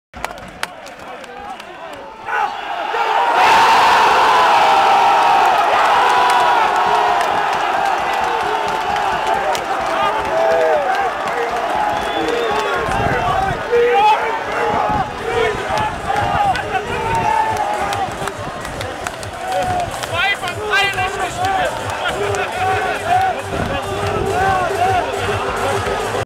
Chemnitzer FC - FC Energie Cottbus II 2:1 (11.02.11)
Tor durch Wilke